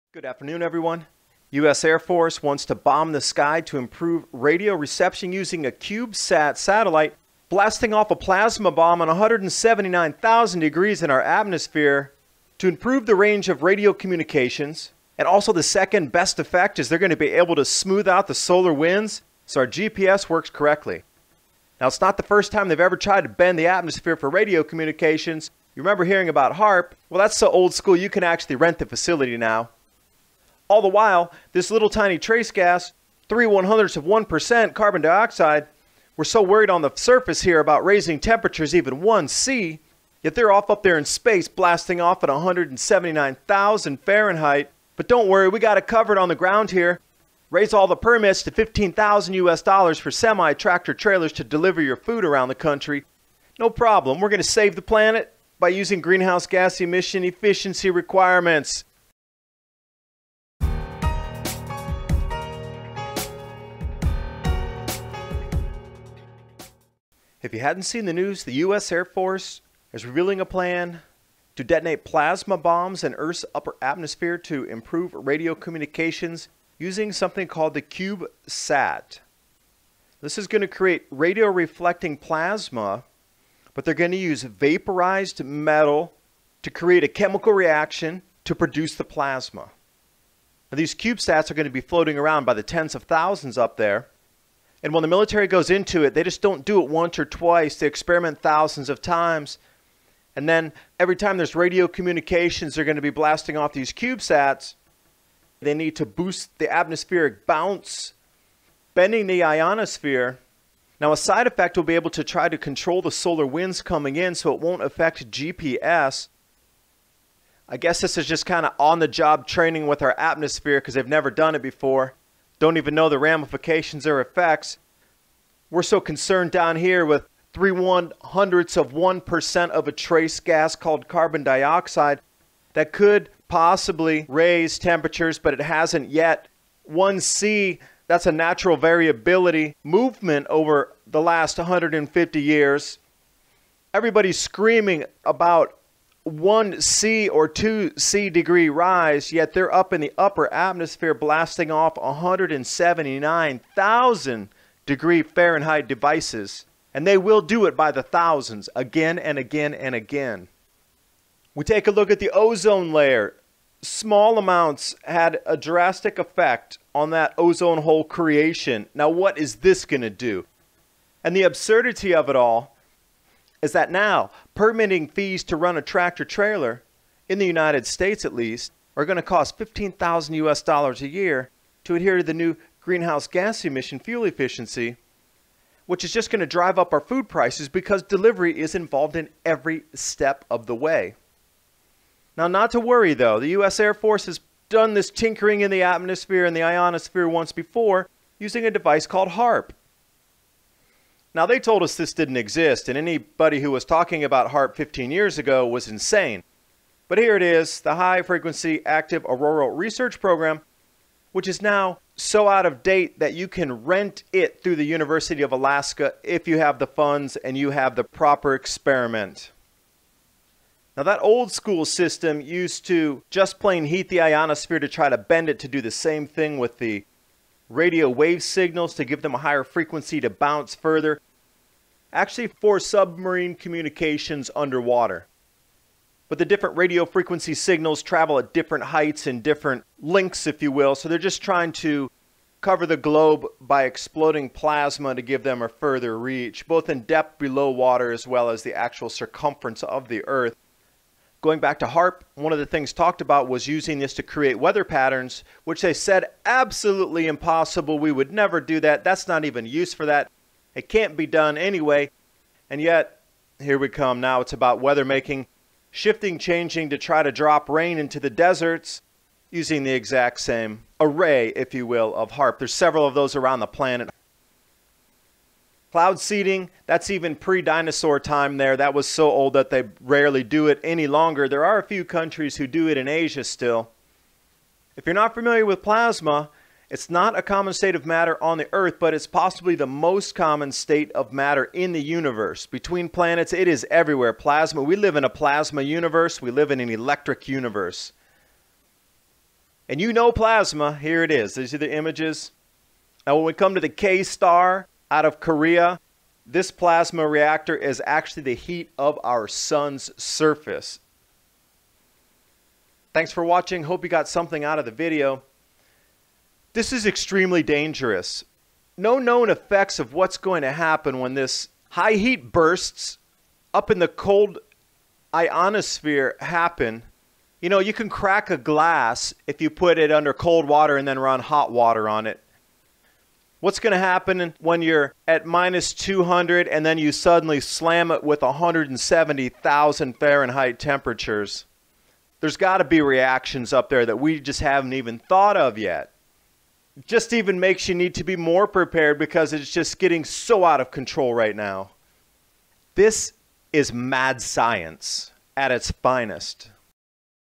Forget CO2 Warming, Plasma Heating in the Atmosphere is the Real Global Danger (209)
The US Air Force has announced the usage of CubeSat a device to create 179,000 F plasma reactions in the upper atmosphere / Ionosphere to improve range of radio communications. Meanwhile we complain and dismantle economies over 2-3 F, based on faulty CO2 models.

Never Pay For Cable TV Again with Tiger Stream!